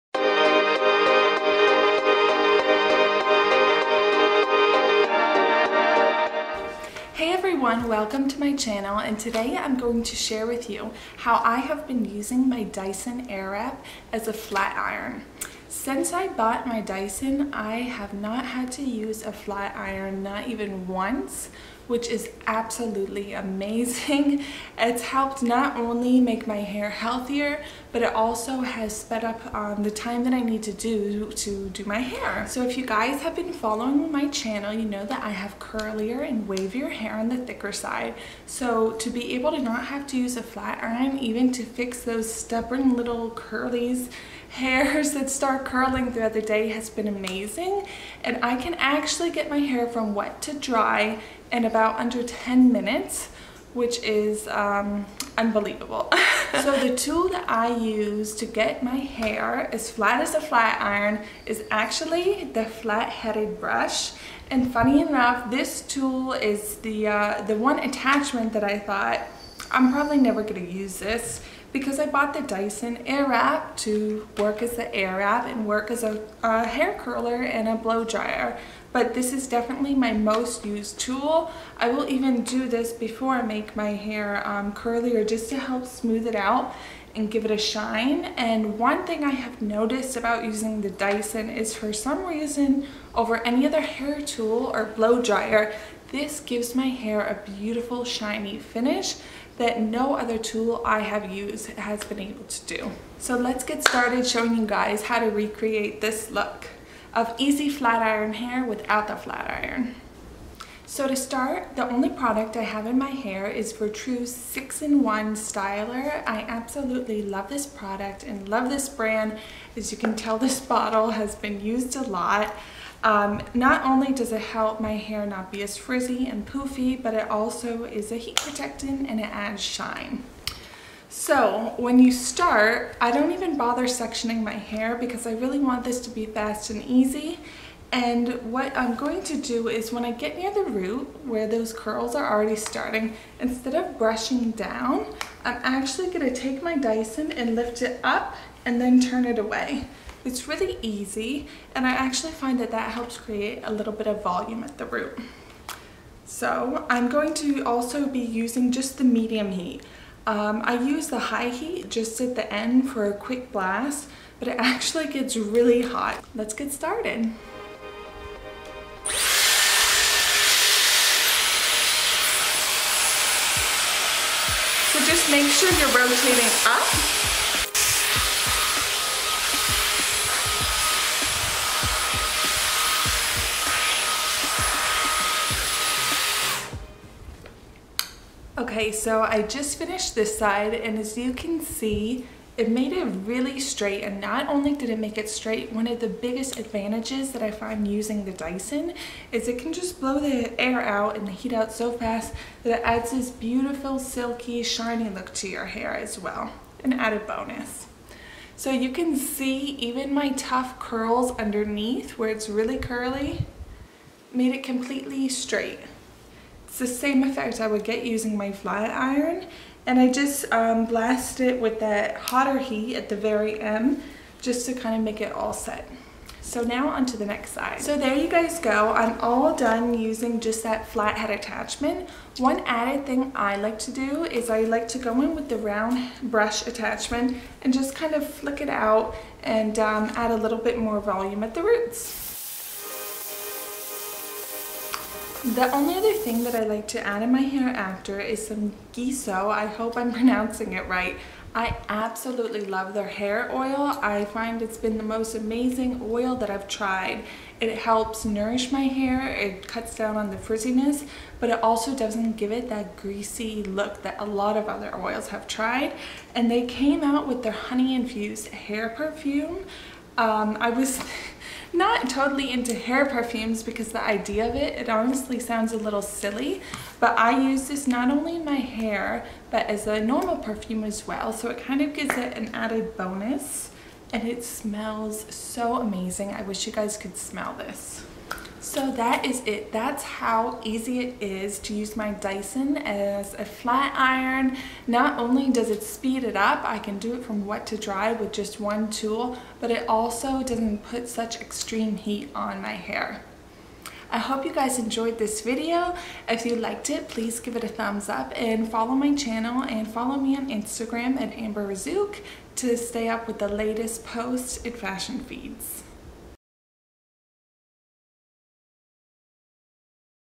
How to use your Dyson Airwrap as a flat iron!
This video is all about how you can get perfectly straight and shiny hair using the Dyson Airwrap! Since I bought my Dyson I have not had to use a flat iron at all, which is amazing. With these tips, you will speed up your hair routine and by using less heat your hair will be thanking you.

Dyson Airwrap complete (this is the one I have)

Dyson smooth set (includes the brush attachments I used)

Virtue Styler